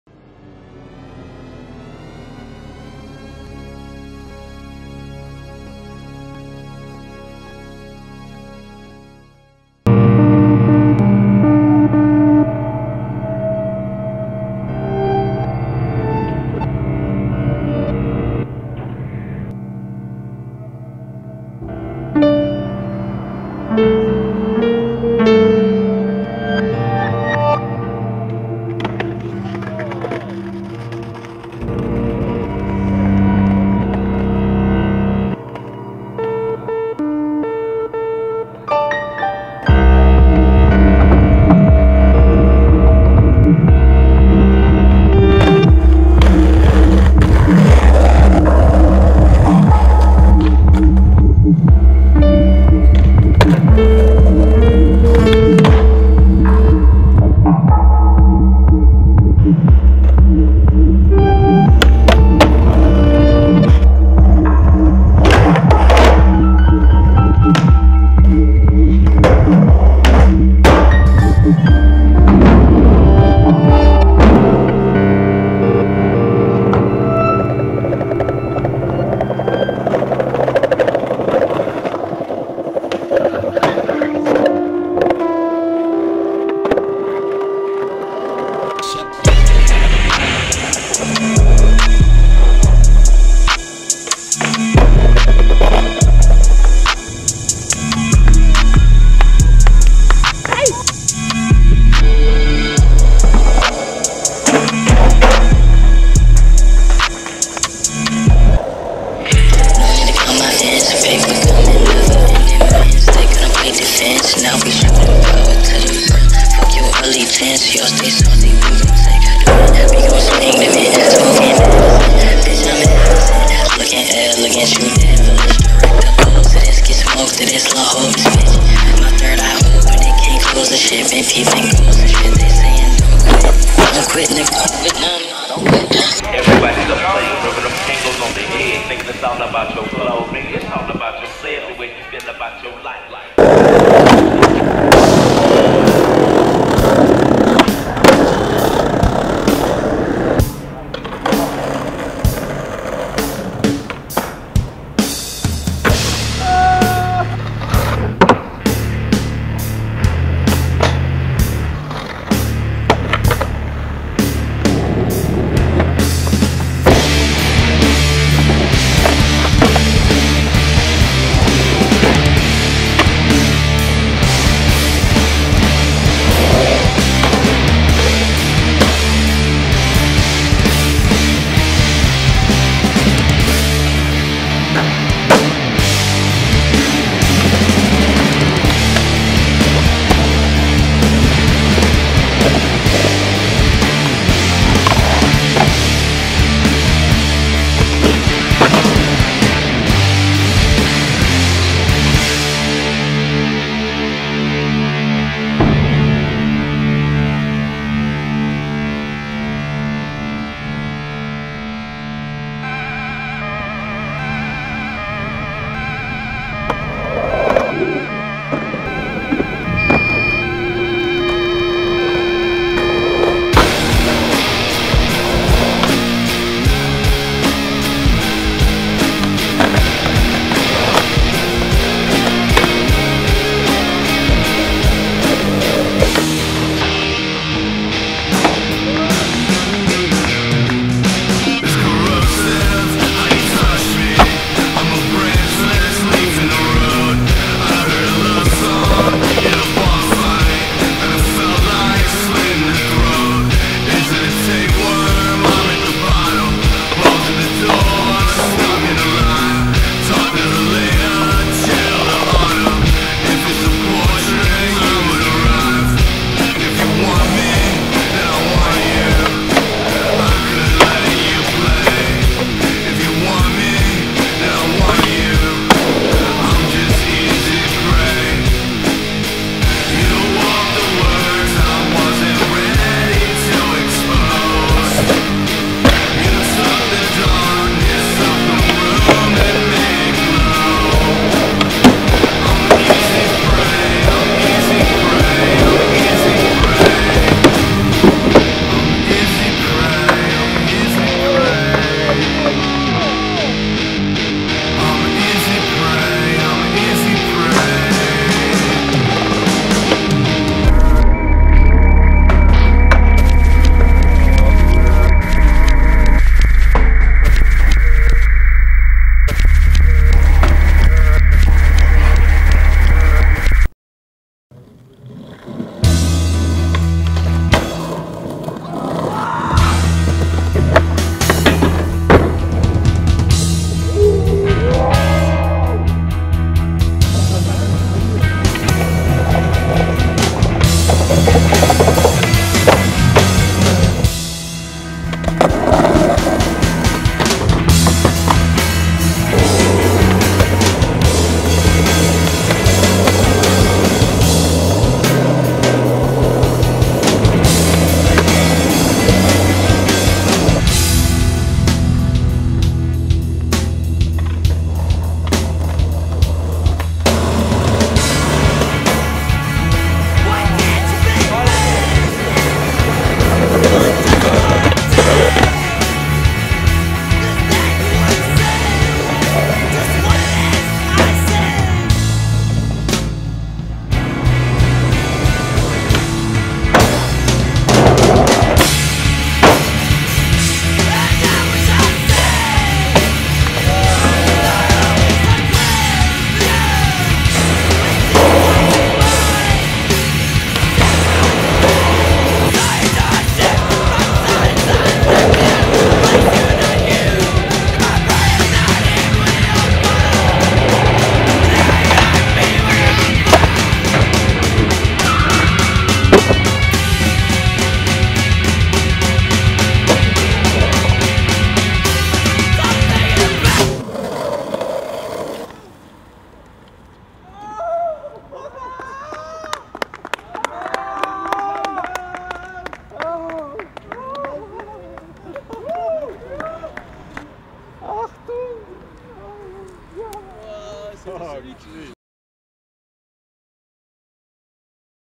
FOUND FOOTAGE
RAVE Skateboards presents Found Footage, starring Alex Richard, Léo Cholet, Mika Germond, Olivier Durou, Edouard Depaz, Paul Austin, PJ Chapuis, Fred Plocque Santos & Amelien Foures.

Filmed and edited by PJ Chapuis with additional footies from good friends Thomas Vigoureux and Luc Mazières.